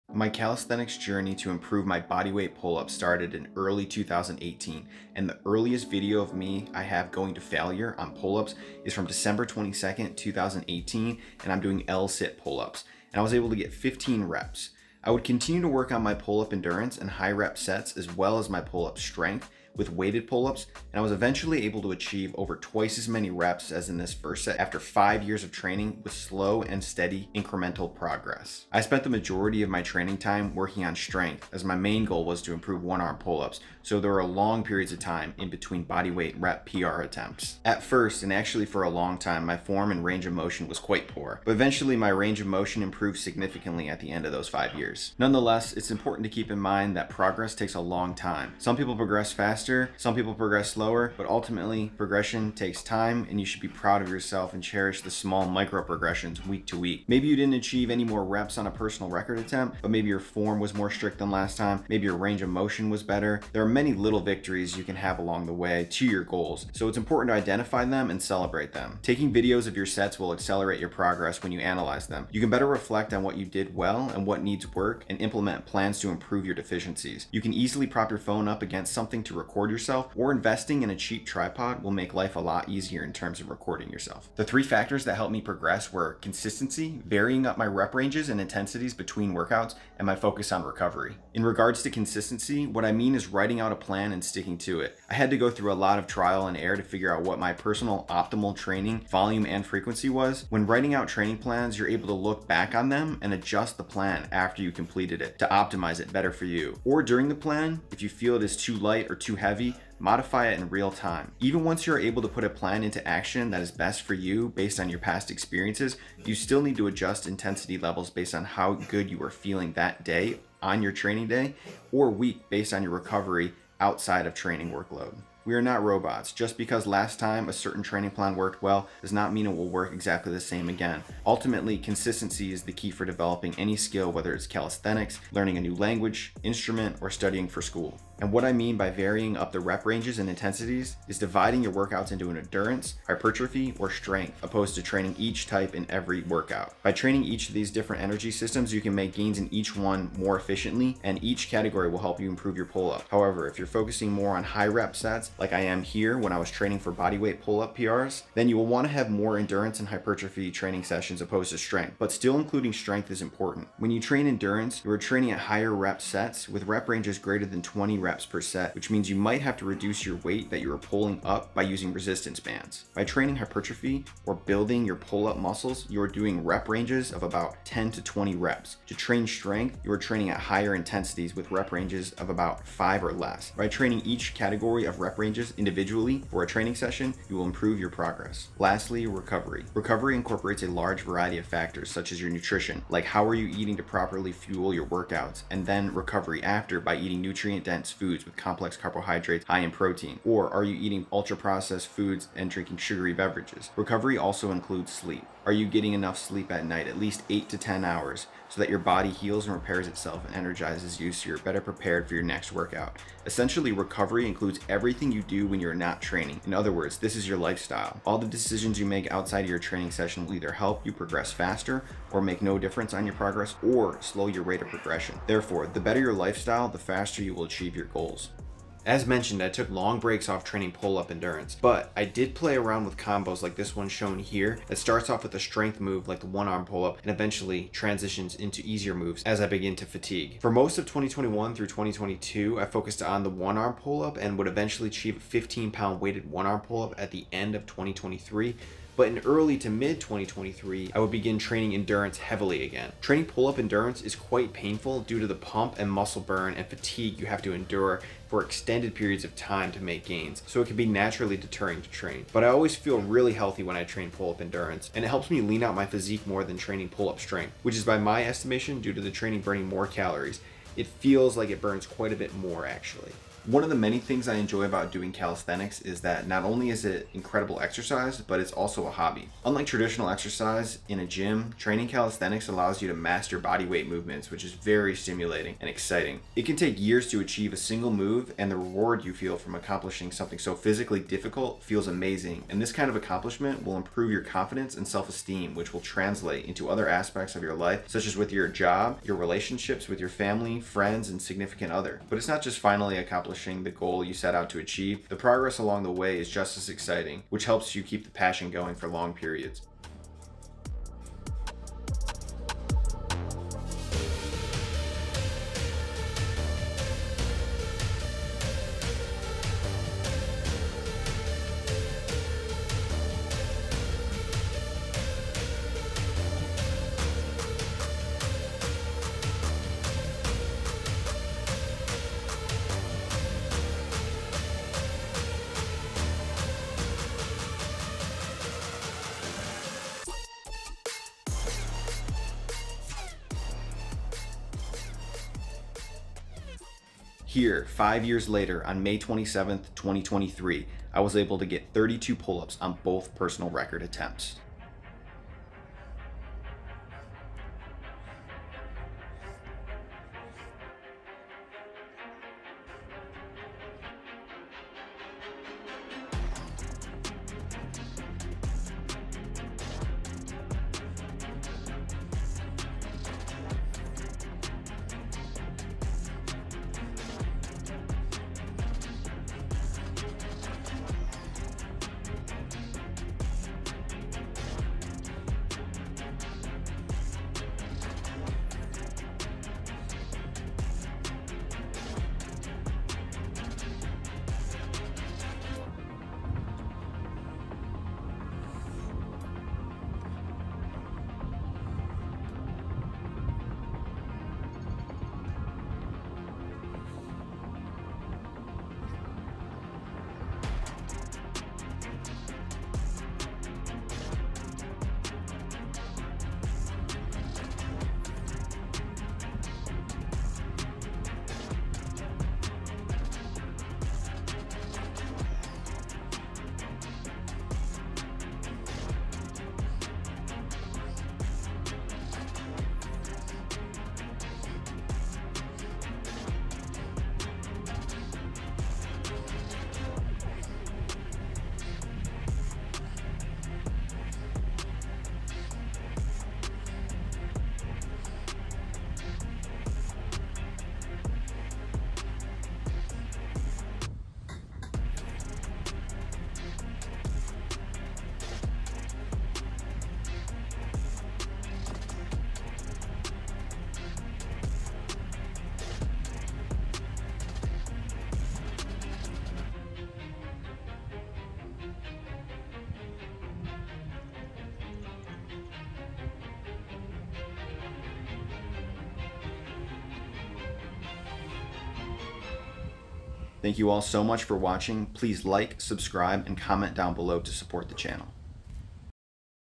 5 Year Pull-Up Progression
Chapters:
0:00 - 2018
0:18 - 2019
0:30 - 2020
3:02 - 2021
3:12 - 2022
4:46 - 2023

My calisthenics journey to improve my bodyweight pull ups started in early 2018 and the earliest video of me I have going to failure on pull ups is from December 22nd 2018 and I am doing L sit pull ups and I was able to get 15 reps. I would continue to work on my pull up endurance with high rep sets as well as pull up strength with weight pull ups and I was eventually able to achieve over twice as many reps as in this first set after 5 years of training with slow and steady incremental progress.
I spent the majority of my training time working on strength as my main goal was to improve my one arm pull up, so there were longer periods in between bodyweight rep PR attempts.
At first and actually for a long time my form and range of motion was quite poor, but eventually my range of motion improved significantly at the end of those 5 years.
Nonetheless, It's important to keep in mind that progress takes a long time. Some people progress faster and some people progress slower but ultimately progress takes time and you should be proud of yourself and cherish the small micro progressions week to week. Maybe you didn't achieve any more reps on a personal record attempt, but maybe your form was more strict than last time and maybe your range of motions was better. There are many little victories that you can have along the way to your goals, so it's important to identify them and celebrate them. Taking videos of your sets will accelerate your progress when you analyze them. You can better reflect on what you did well and what needs work and implement plans to improve your deficiencies. You can easily prop your phone up against things to record yourself or invest in a cheap tripod.
Three factors that helped me progress were consistency, varying my rep ranges and intensities, and my focus on recovery.
In regards to consistency, what I mean is writing out a plan and sticking to it. I had to go through a lot of trial and error to figure out what my personal optimal training volume and frequency was. writing out training plans you are then able to look back and adjust the plan after you completed it to optimize it better for you, or during the plan if you feel it is too light or too heavy, modify it in real time. Even once you are able to put a plan into action that is best for you based on your past experiences you still need to adjust intensity levels based on how you are feeling that day and week based on your recovery and outside training workload. We are not robots just because last time a certain training plan worked well does not mean it will work exactly well the same again. Ultimately, consistency is the key for developing any skill whether it is calisthenics, learning a new language, an instrument, or studying for school.
And what I mean by varying up the rep range and intensity is dividing your workouts into endurance, hypertrophy, or strength opposed to training each type in every workout. Buy training these different energy systems you can make gains in each one more effectively and each category will help improve your pull ups, however, if you are focusing more on higher rep sets, like I am here,  then you will want to have more endurance and hypertrophy training sessions opposed to strength, but still including strength is important. When you train endurance you are training at higher reps sets with rep ranges greater than 20 reps per set, which means you might have to reduce your weight that you are pulling up by using resistance bands. By training hypertrophy or building you pull up muscles you are doing rep ranges of about 10-20. To train strength you are training in high intensities with rep ranges about 5 or less. By training each category of rep ranges you will improve your progress.
Lastly, recovery. Recovery incorporates a large variety of factors such as your nutrition, like are you eating properly to fuel your workouts and then recovery after by eating nutrient dense foods with complex carbohydrates or are eating ultra processed foods and sugary beverages. recovery also includes sleep. Getting enough sleep at 8-10 hours per night, so that your body heals and repairs itself and energizes you so you are better prepared for your next workout. Essentially recovery includes everything you do when you are not training, in other words this is your lifestyle. All the decisions you make outside of your training session will either help you progress faster or make no difference on your progression or slow your rate of progression. Therefore the better your lifestyle the faster you will achieve your goals.
Here 5 years later on May 27th 2023 I was able to get 32 pull ups on both personal record attempts.